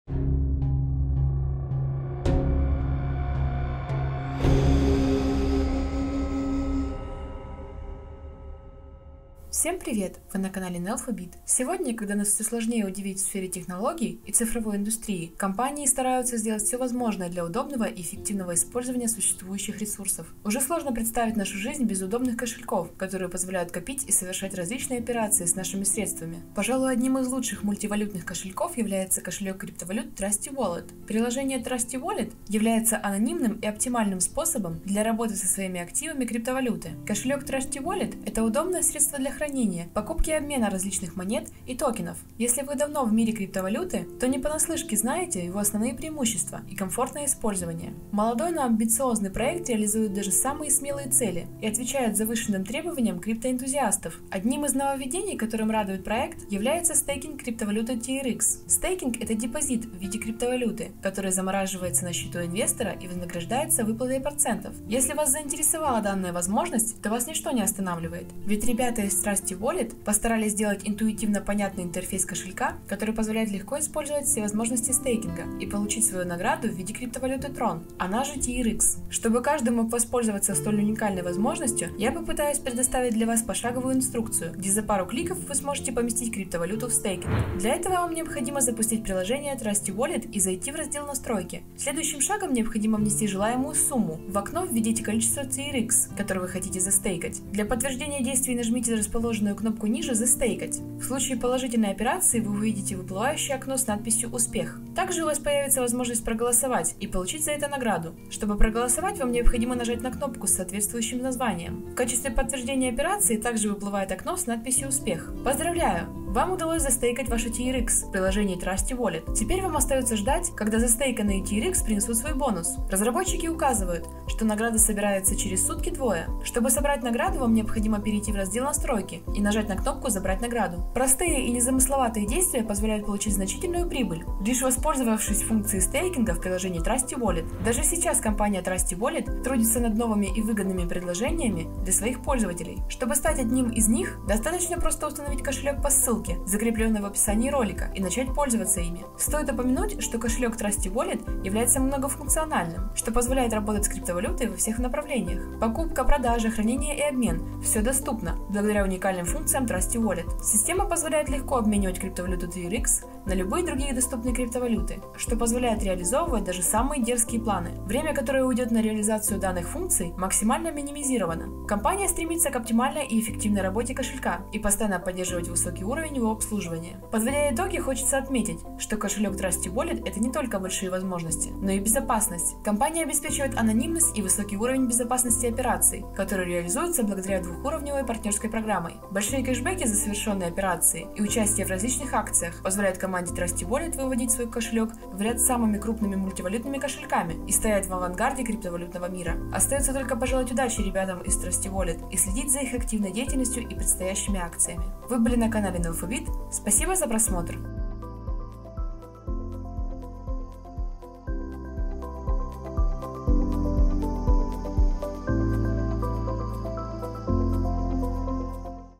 💎TRUSTEE WALLET – Стейкинг Уже в Приложении!
Приложение Trustee Wallet – является анонимным и оптимальным способом для работы со своими активами криптовалюты. Кошелек Trustee Wallet – это удобное средство для хранения, покупки и обмена различных монет и токенов.

Если вы давно в мире криптовалюты, то не понаслышке знаете его основные преимущества и комфортное использование. Молодой, но амбициозный проект реализует даже самые смелые цели и отвечает завышенным требованиям крипто-энтузиастов. Одним из нововведений, которым радует проект является Стейкинг криптовалюты.

📄 Ethereum – 0x64e5a12DA2DE76280dDA7Fe274d1A129155Ba06a
📄 TRX (TRON) – TBzboNkRB6DGTdNf4dQMnvCyKXDCHfrTRU